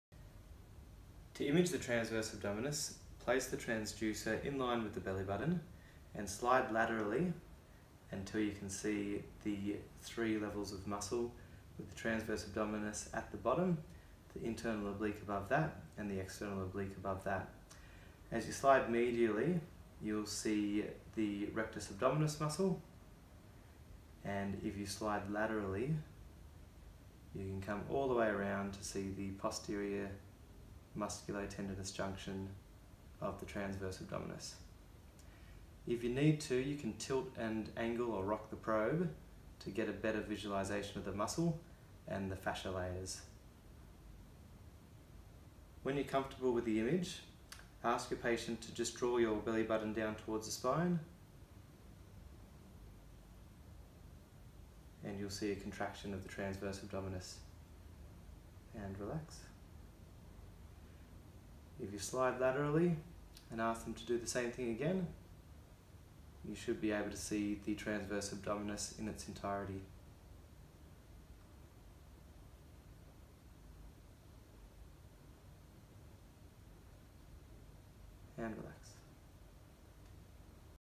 Imaging Tra with QSONO D8
Physiotherapists often prescribe exercises to strengthen the "core" muscles, inclduing the transverse abdominis muscles. Real time ultrasound can be a great way to check the muscle activation of the transverse abdominis and other core muscles. It is also a really helpful tool for showing patients the muscle and helping them understand how it functions.

In this video we show how to obtain an image of the transversus abdominis muscle using real time ultrasound. The ultrasound machine used in this video is the handheld wireless QSONO D8 ultrasound, it is paired with an iPad.
The transducer is placed in a transverse orientation in the mid axillary line, at the height of the umbilicus. The transverse abdominis can be visualised as the 3rd layer of muscle, beneath the internal and external oblique muscles.